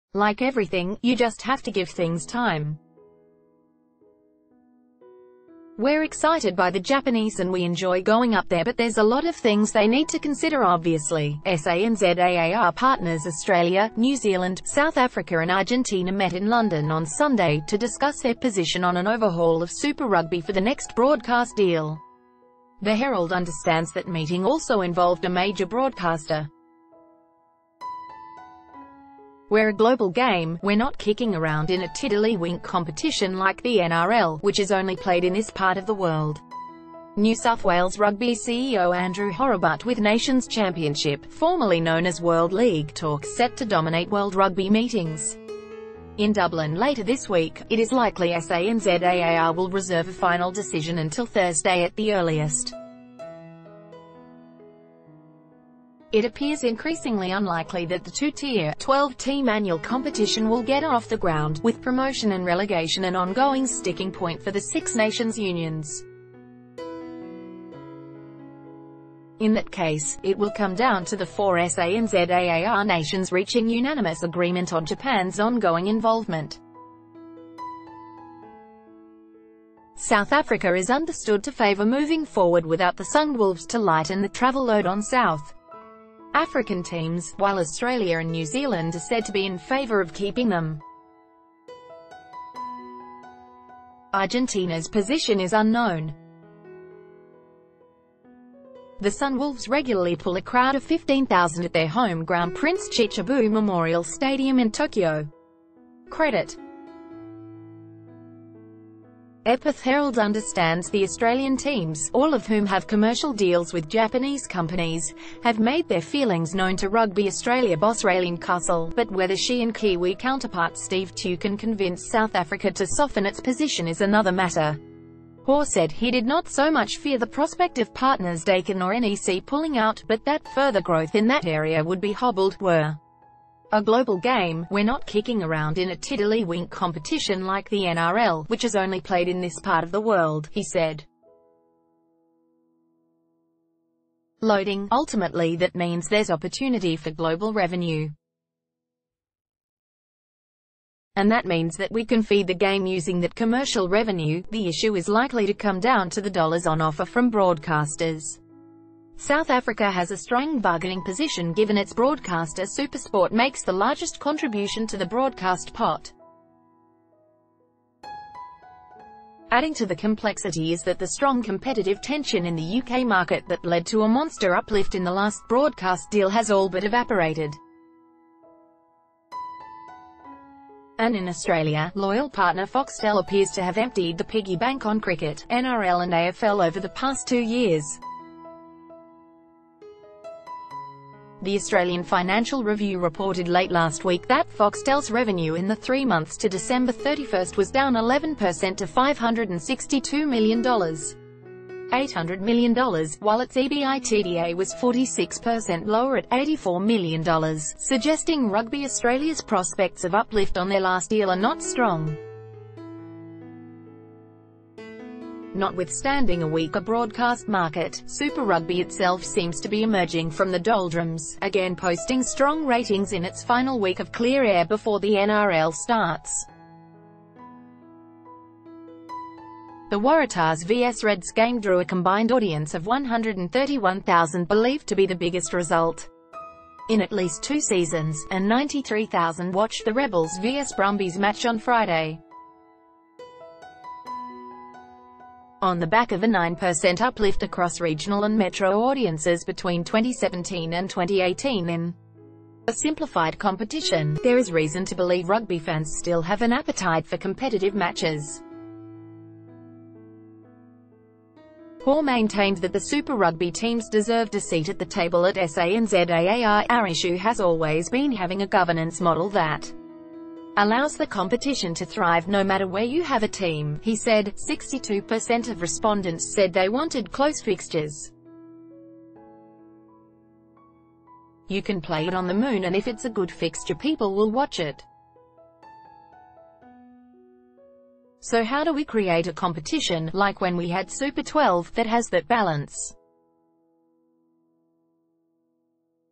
Super Rugby 2019: Waratahs boss urges SANZAAR to save Sunwolves
"Like everything, you just have to give things time. We’re excited by the Japanese and we enjoy going up there but there’s a lot of things they need to consider obviously."SANZAAR partners Australia, New Zealand, South Africa and Argentina met in London on Sunday to discuss their position on an over...